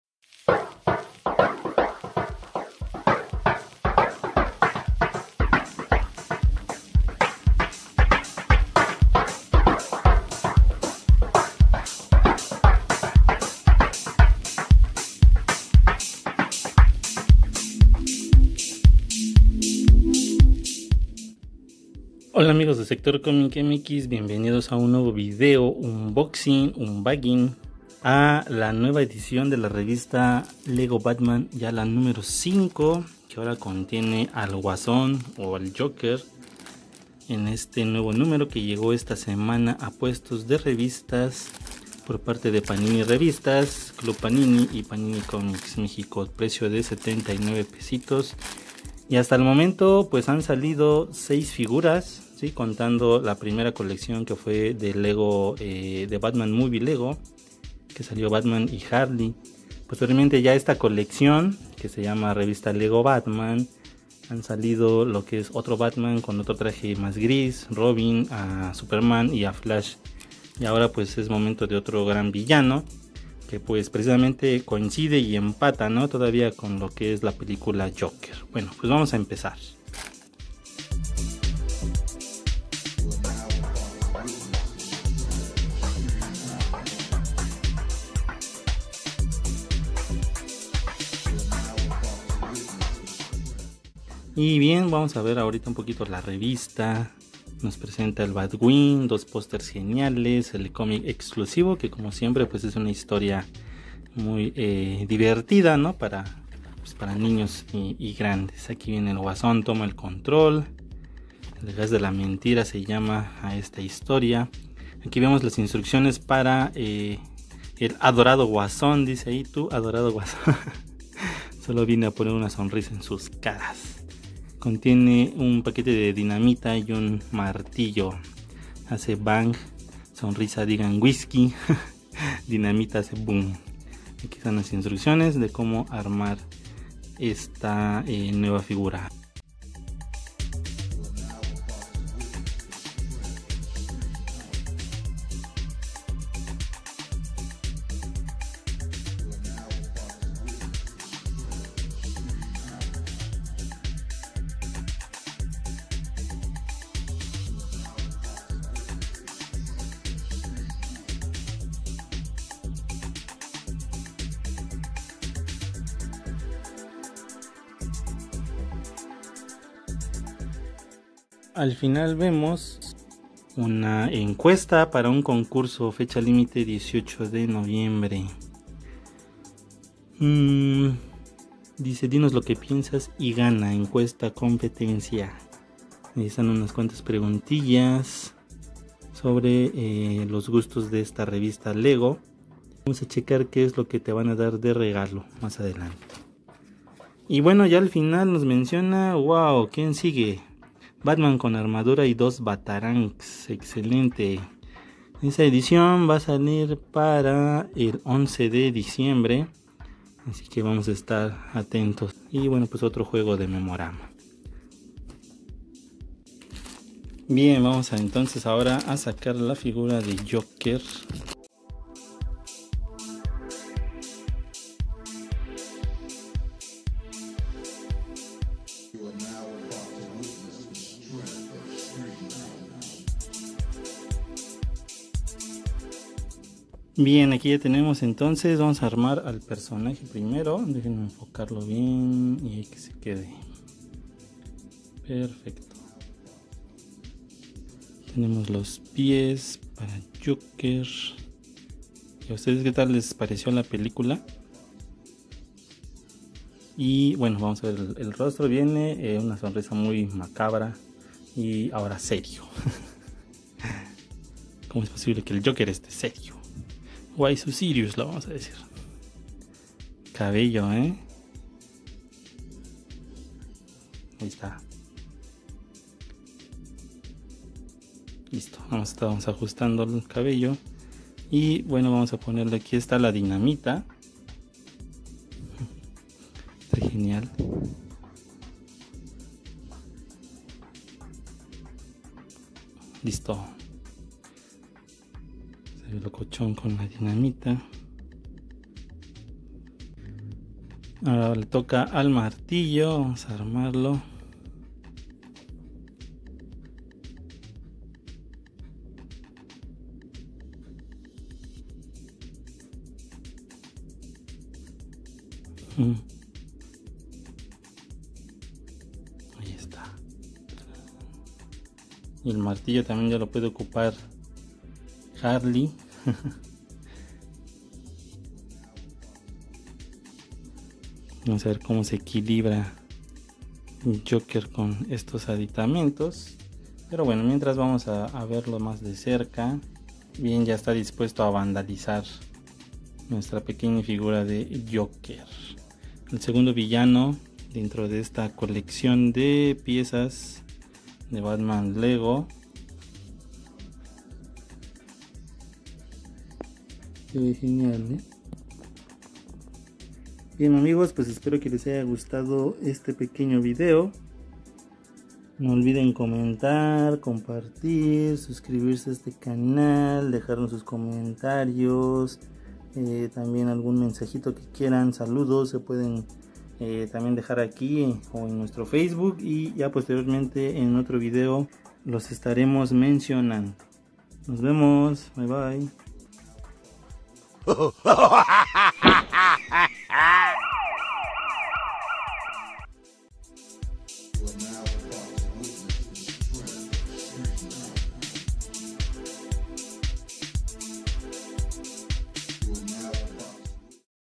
Unbagging a The Joker en la Revista Lego Batman #5 de Panini Revistas
La figura Lego de El Guasón llega en el mas reciente número de la Revista Lego Batman.